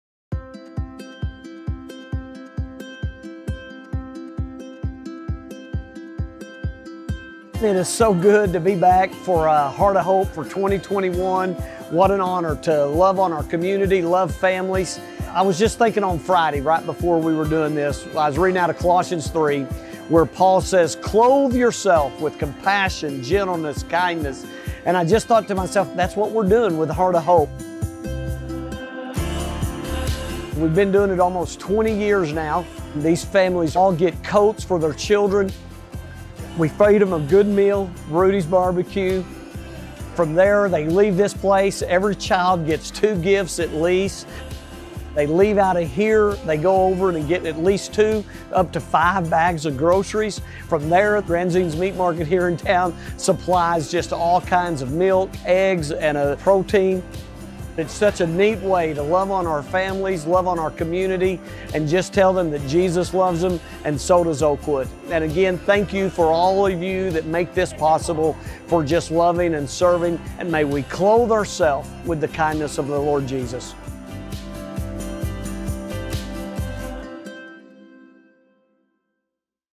Heart of Hope 2021
What a great day of service to the Lord! We want to extend a big thank you to all who helped with Heart of Hope in various ways of prayer, donations of time, resources, and talents, and showing love to our community. We were able to intentionally engage and serve over 800 guests!